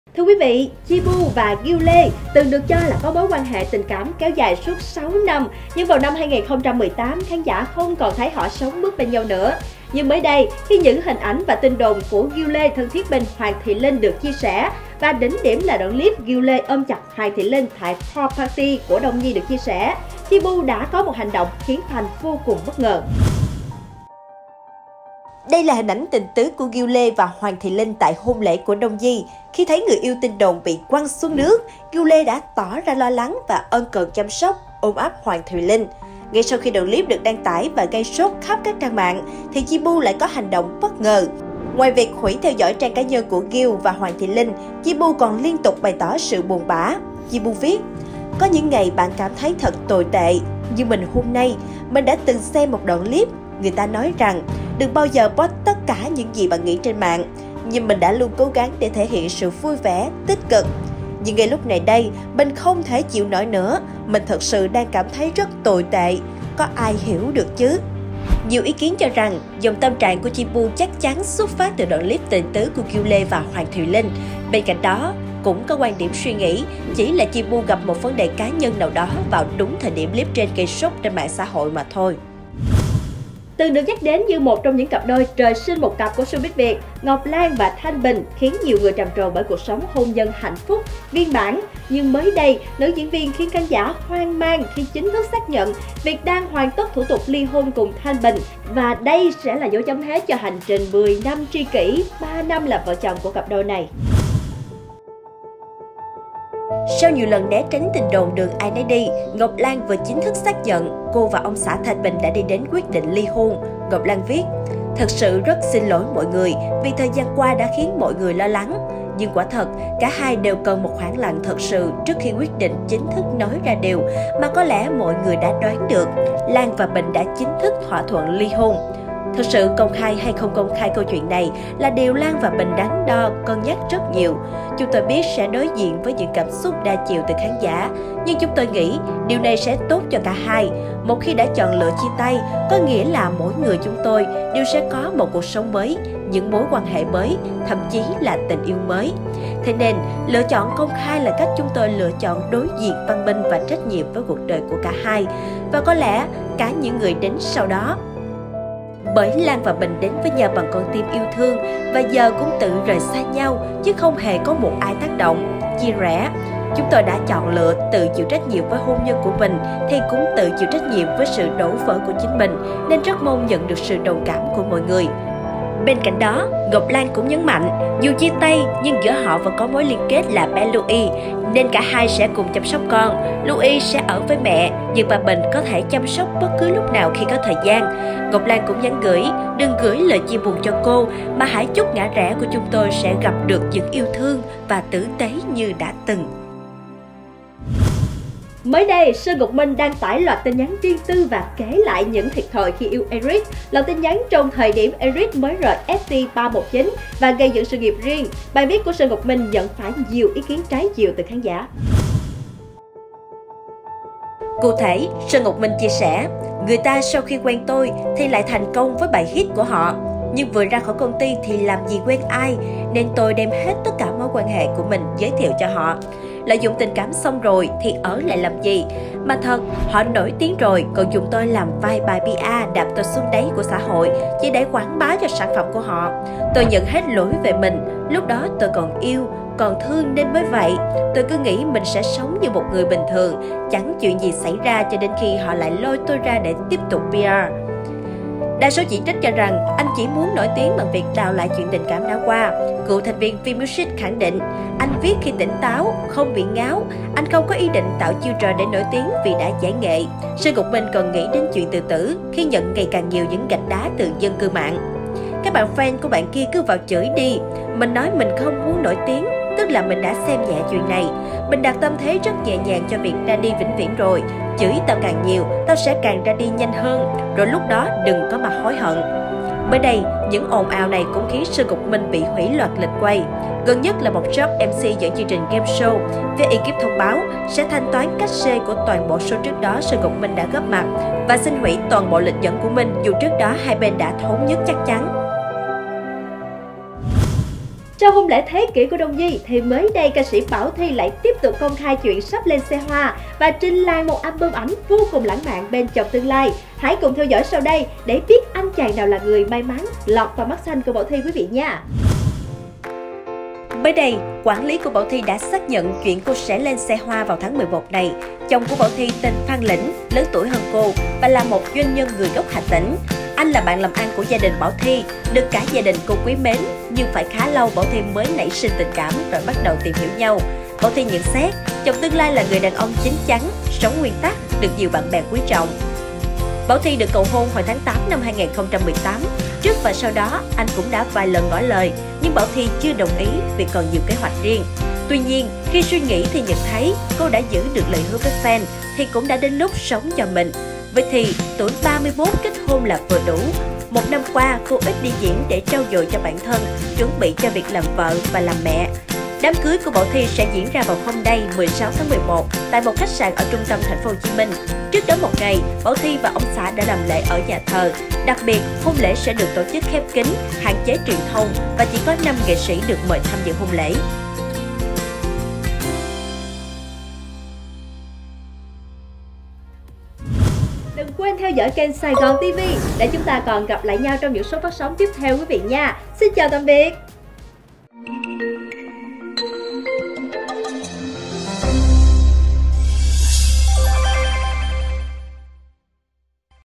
Chi Pu lên tiếng về status nghi liên quan đến Gil Lê và Hoàng Thùy Linh
Ngày 13/11, Chi Pu có chia sẻ đầy tâm trạng và còn hủy theo dõi Gil Lê - Hoàng Thùy Linh. Nhiều người cho đây là động thái của cô sau khi đoạn clip Gil Lê - Hoàng Thùy Linh ôm nhau thân mật ở lễ cưới đông Nhi lan truyền trên mạng xã hội. Tối 13/11, Chi Pu lên tiếng.

Xem thêm thông tin và theo dõi chúng tôi tại